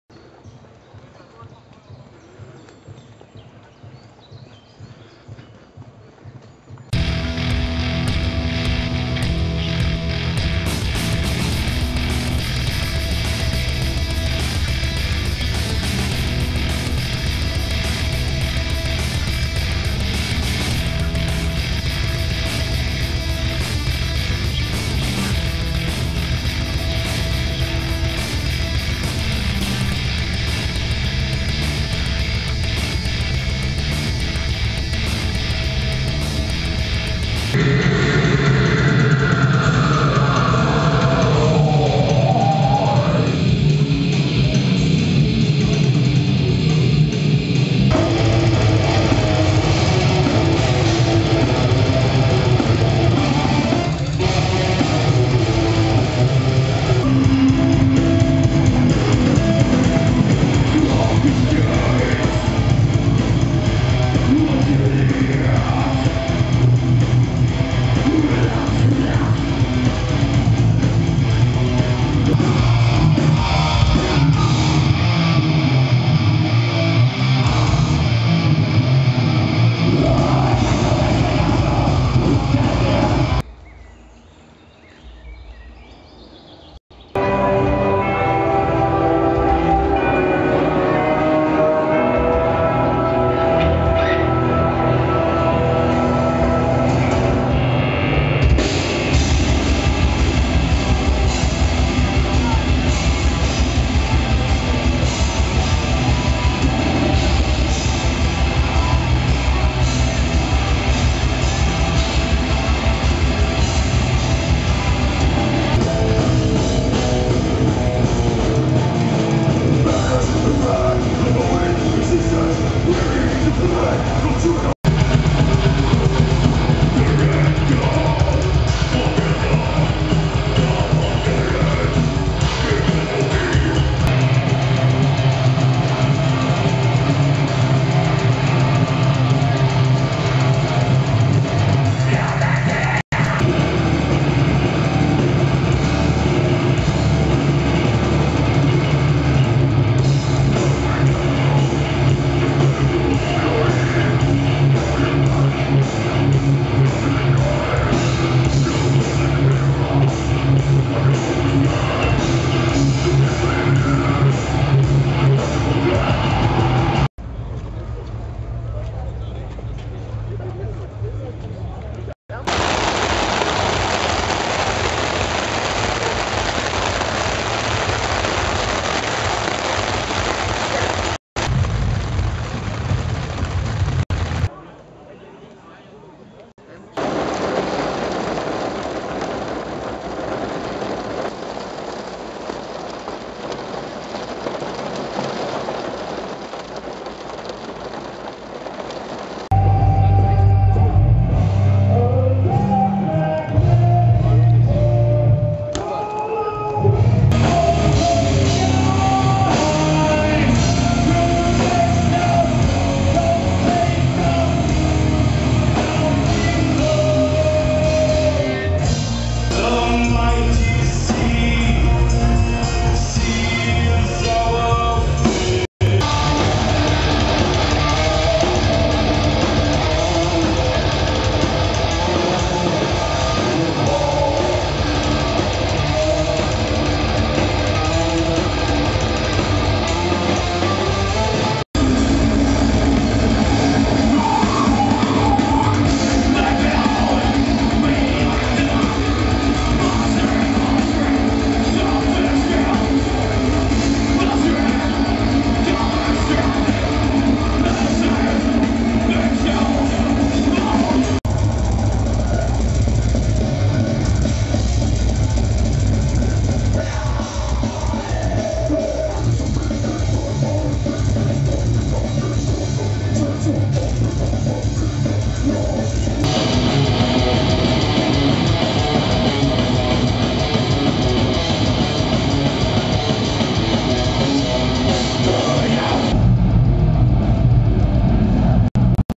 TAGEBUCH: Metalfestival Chronical Moshers 2015.
Wenn ich nicht gerade Bücher lese, höre ich gern menschenverachtende Musik. ♥ Nicht mein üblicher Videoinhalt - also Vorsicht für die Metalfernen unter euch: Hier kommt Death, Thrash und Black aus Hauptmannsgrün.